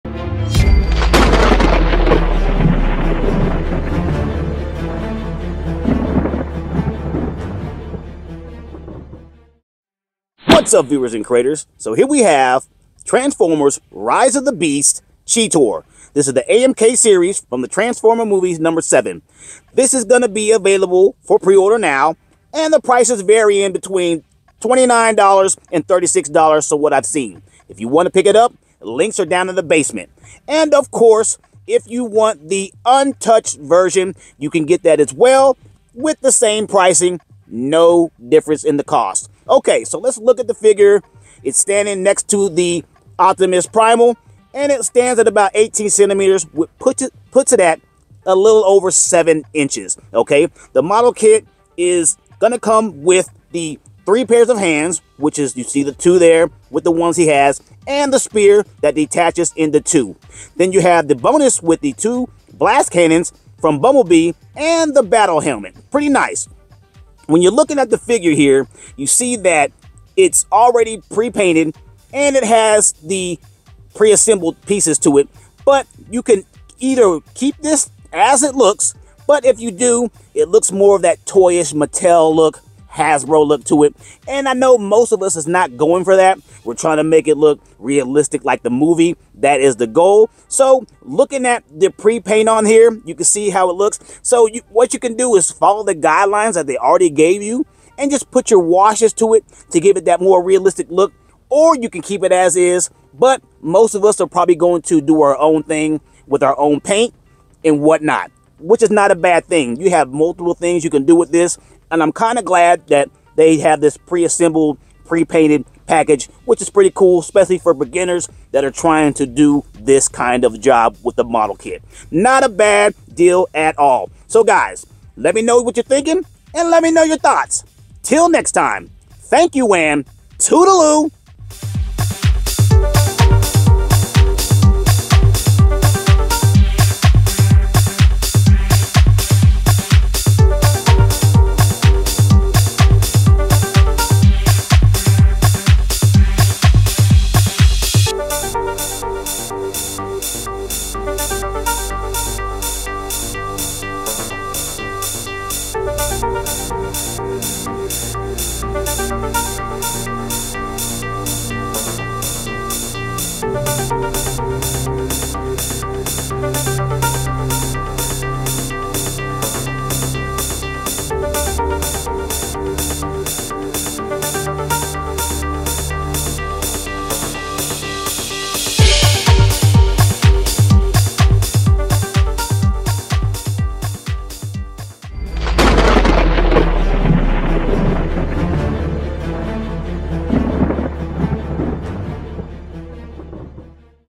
Yolopark AMK Series Transformers Rise Of The Beasts Movie 7 CHEETOR In-Hand Images(Pre-order)
Yolopark AMK Series Transformers Rise Of The Beasts CHEETOR In-Hand Images(Pre-order)

SLIDE SHOW:
Promotion Photos

ATTENTION: *we don't not own the rights to some visuals or music*